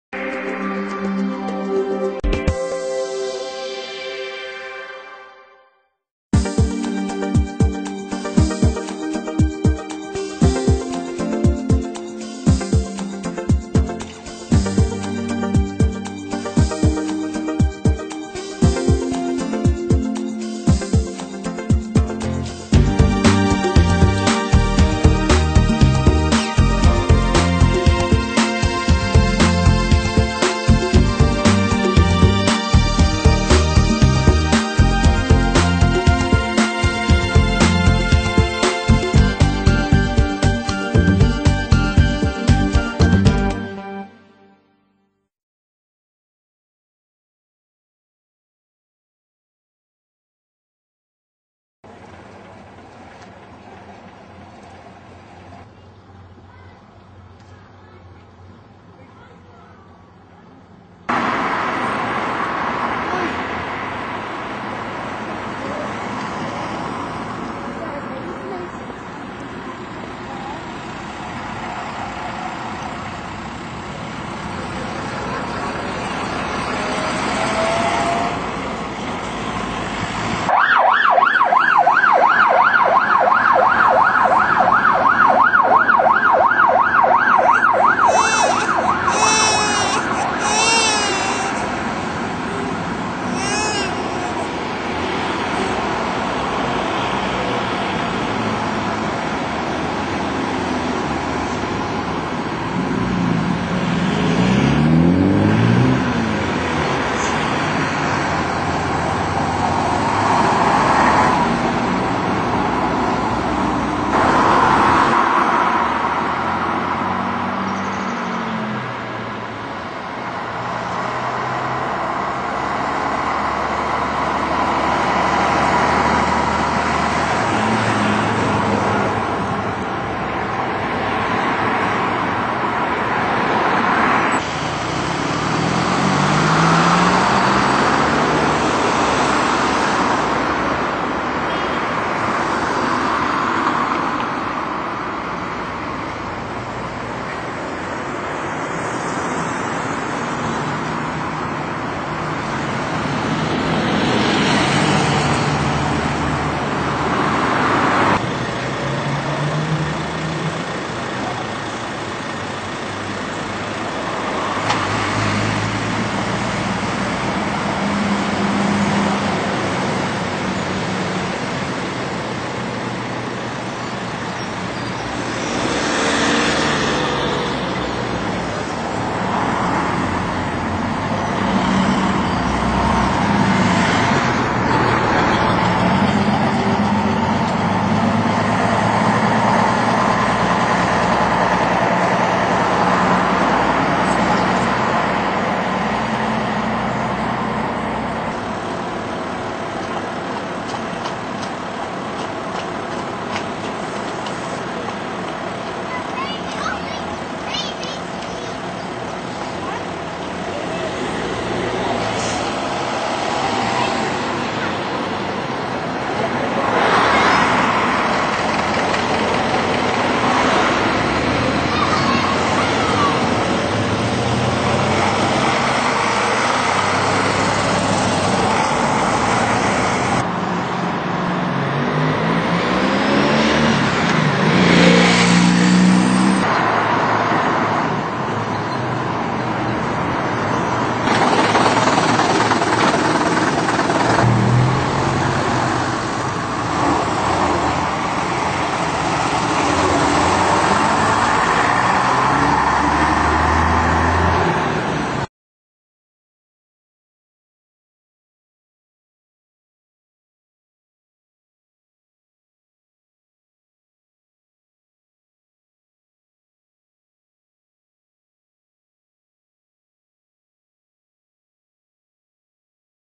SOUTH CHESHIRE Classic Bus Running Day 2009
This its, second event is the South Cheshire Classic Bus Running Day 2009 held at the Crewe Heritage centre and Bus station on the 5th April 2009. This event is highly recommended if you want to visit next year.